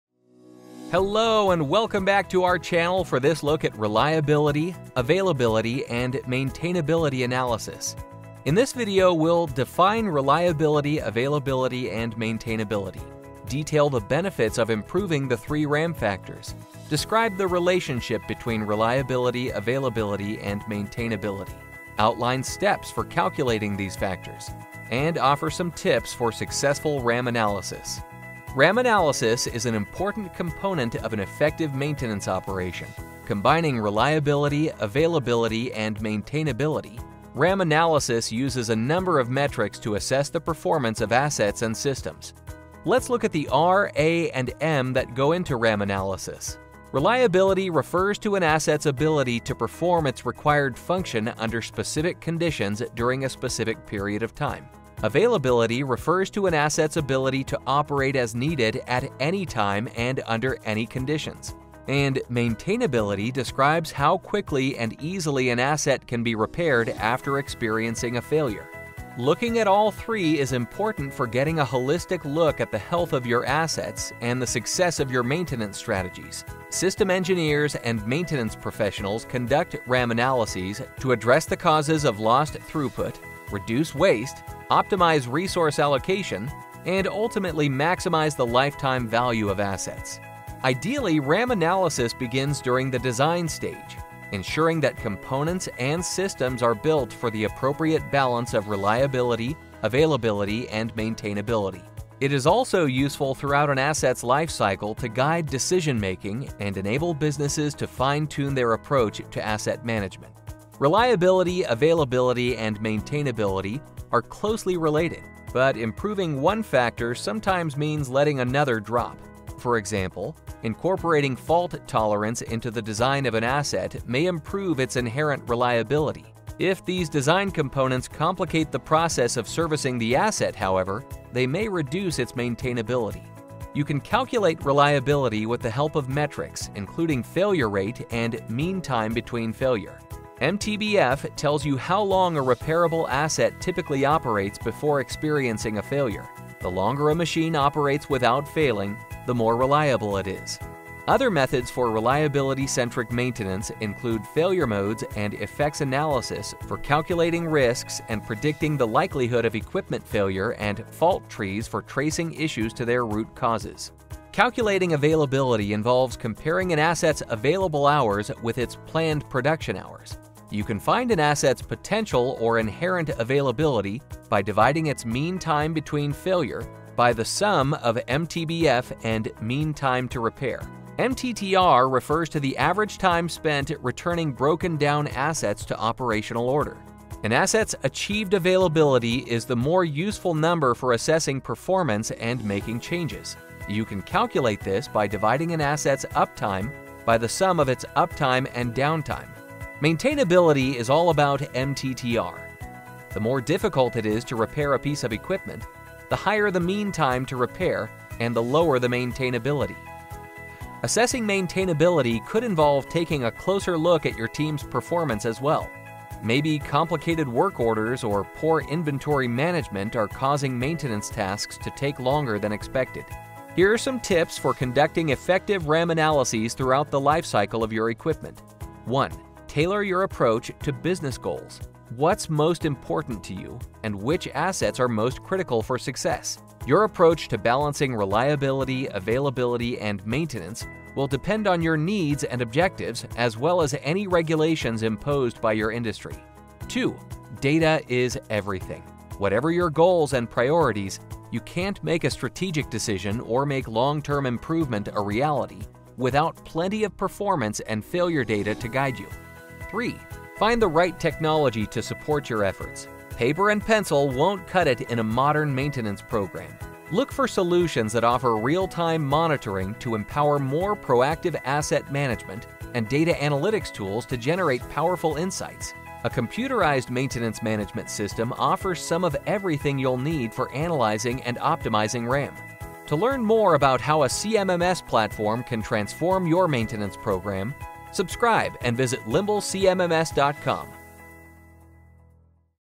Explained: Reliability, Availability, Maintainability (RAM)
In this video, we’ll:

Define Reliability, Availability, and Maintainability
Detail the benefits of improving the three RAM factors
Describe the relationship between reliability, availability, and maintainability
Outline steps for calculating these factors
And offer some tips for successful RAM analysis